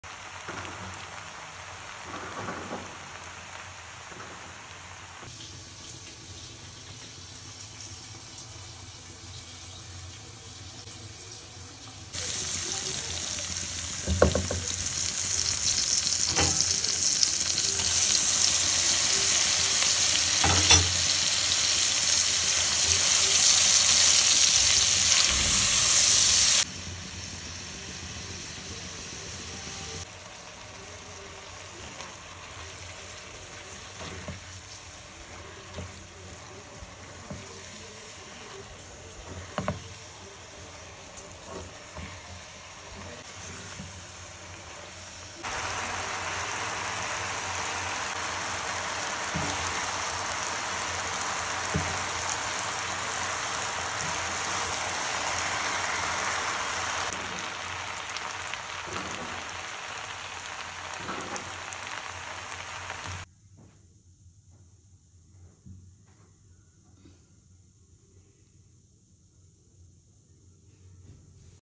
How to make Restaurant Style Chicken Spaghetti | Spaghetti at home
Hello everyone!

Today I’m sharing my easy Recipe of making restaurant style Chicken and Vegetable Spaghetti/ Noodles with you all ❤️

Do try it out and if you do, please share your feedback too! 🤌🏻